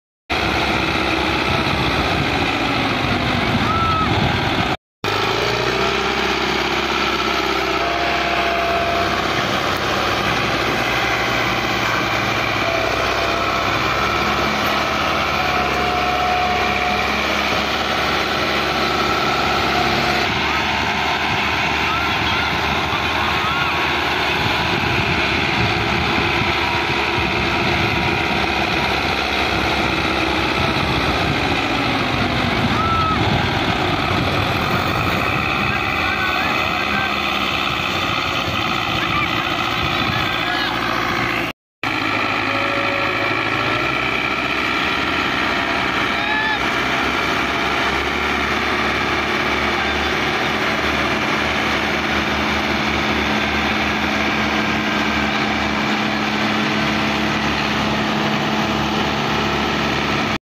Belarus 510 Tractor Dangerous Climbing Trolley Full Of Sugar Cane | Belarus Tractor Fail
Real Tractor Show 🚜🙏🏻